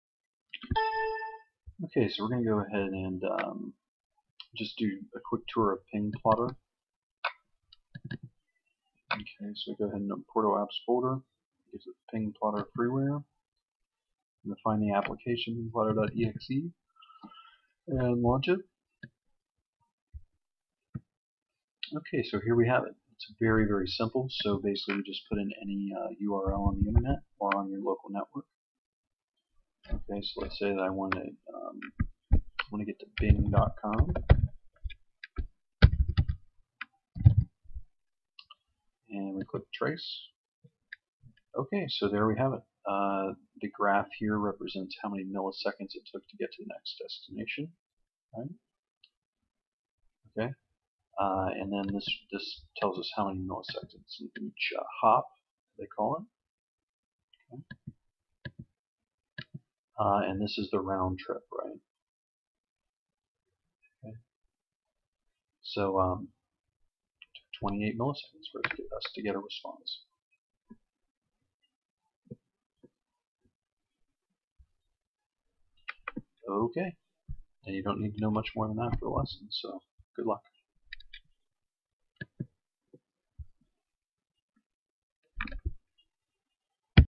How to launch and use basic features of Ping Plotter Lesson Tutorial
How to Use Ping Plotter Tutorial Lesson shows how to use this network sniffer software . It includes launching and use of the basic feature set of this portable application.